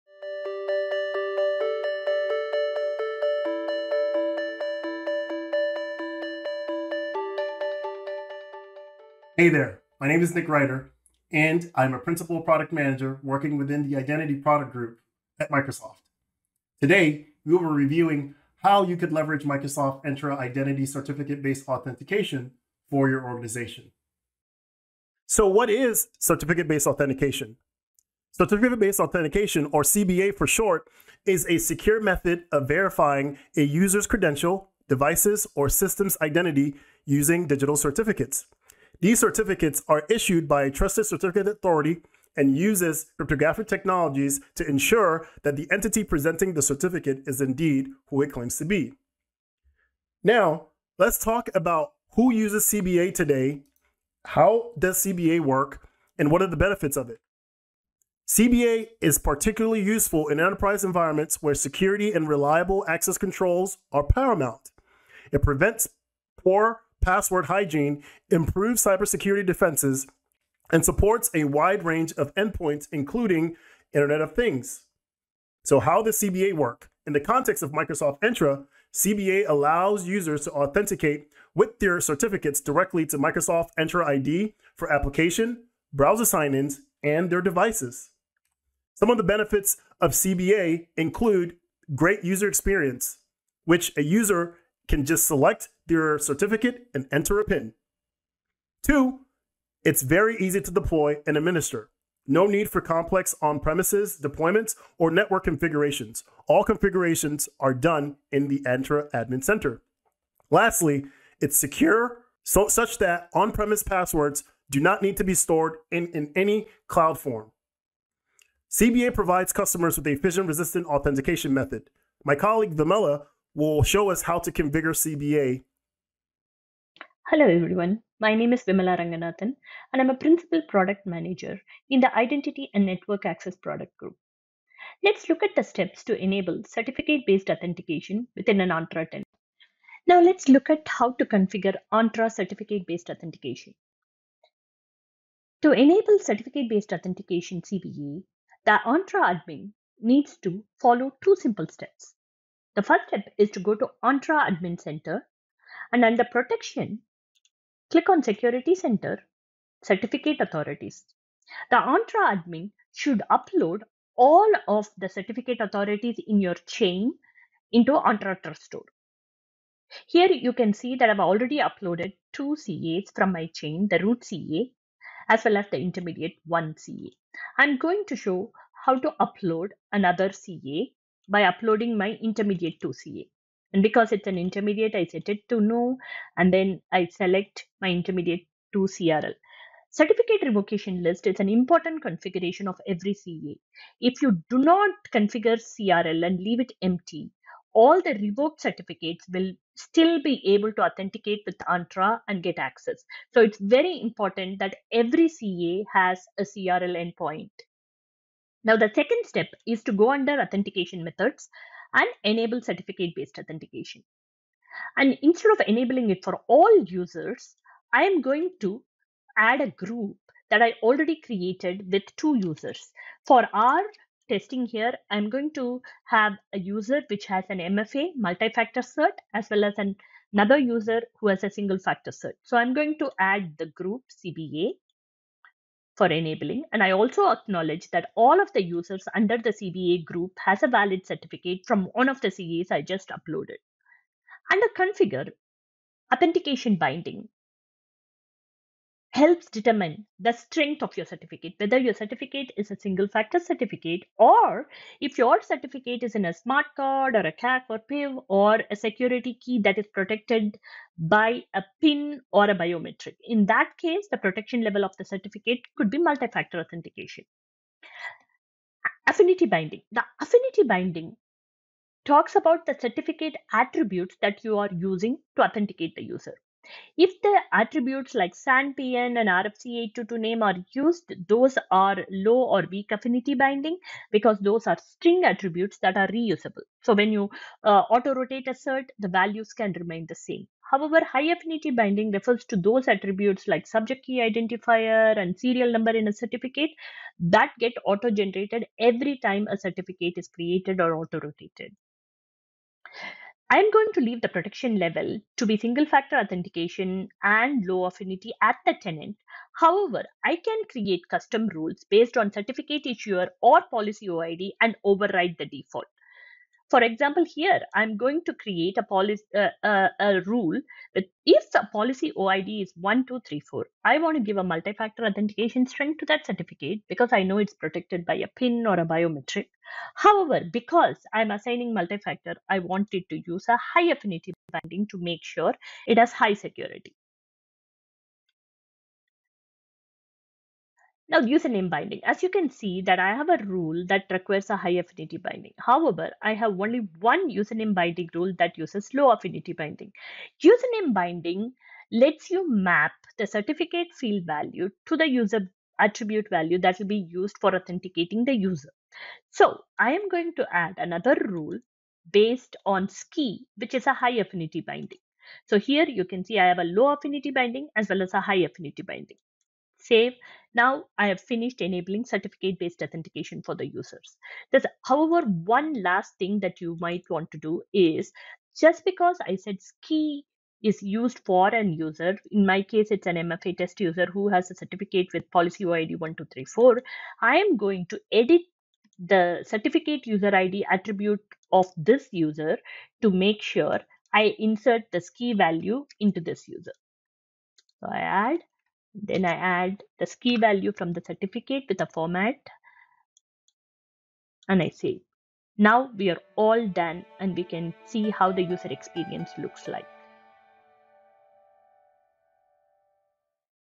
Configure Microsoft Certificate Based Authentication
Learn what is Microsoft Entra Certificate Based Authentication. In this video we show you step-by-step how to configure Microsoft Entra Certificate Based Authentication in your tenant.